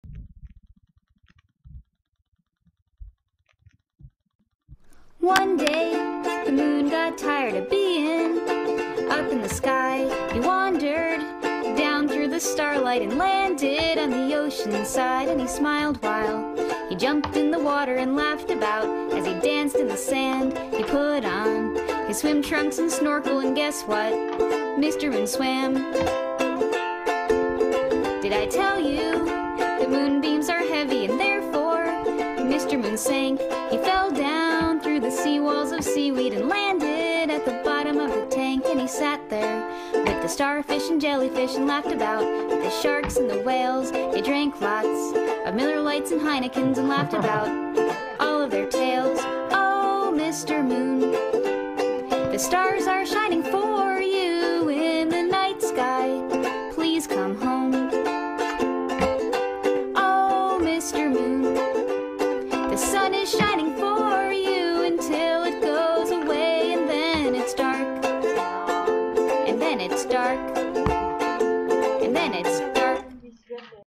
Webcam manœuvre
fingerboard on 24 december before xmas fun sesh filming with my laptop